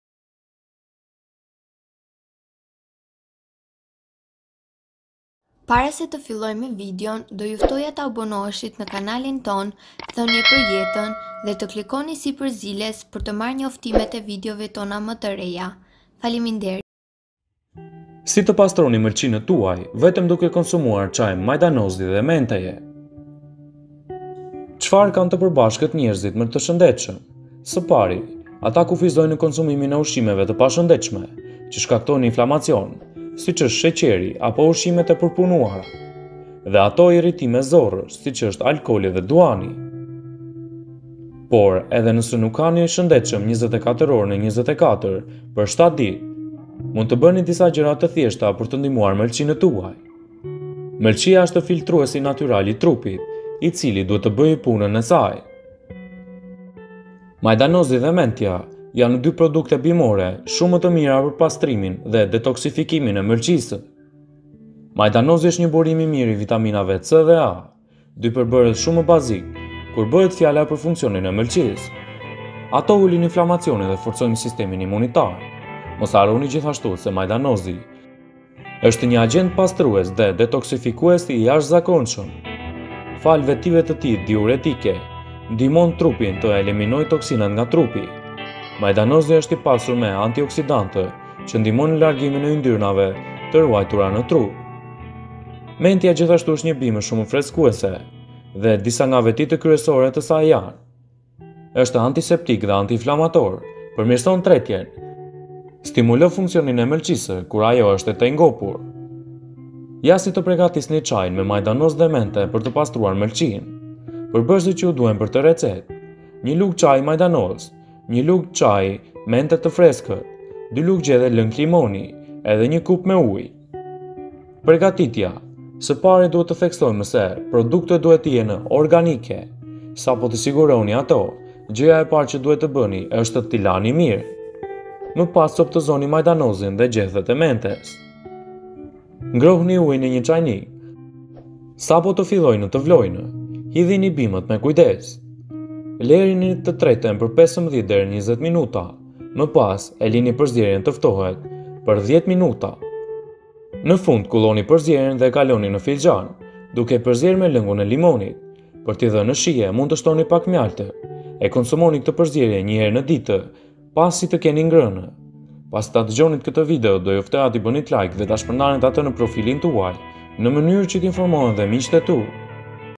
Si te pastroni melcine tuaj vetem duke konsumuar kete caj
Majdanozi dhe mentja janë dy produkte bimore shumë të mira për pastrimin dhe detoksifikimin e mëlçisë.Majdanozi është një burim i mirë i vitaminës C dhe A, dy përbërës shumë bazikë kur bëhet fjalë për funksionin e mëlçisë. Ato ulin inflamacionin dhe forcojnë sistemin imunitar. Mos harroni gjithashtu se majdanozi është një agjent pastrues dhe detoksifikues i jashtëzakonshëm.

Parsley and mint are two very good herbal products for cleansing and detoxifying the liver. Parsley is a good source of vitamins C and A, two very basic ingredients when it comes to liver function. They reduce inflammation and strengthen the immune system. Remember also that parsley is an extraordinary cleansing and detoxifying agent.